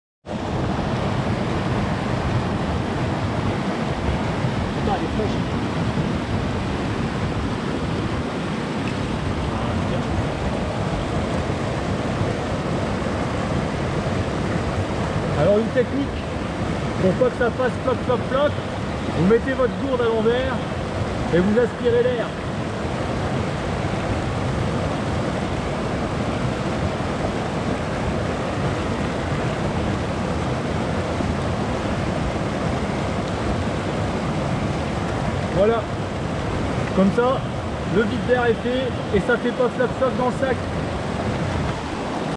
enlever le bruit de la poche à eau (Trail et Rando)
Vous souhaitez ne plus entendre ce bruit agaçant de votre poche à eau.
Voici une technique très simple.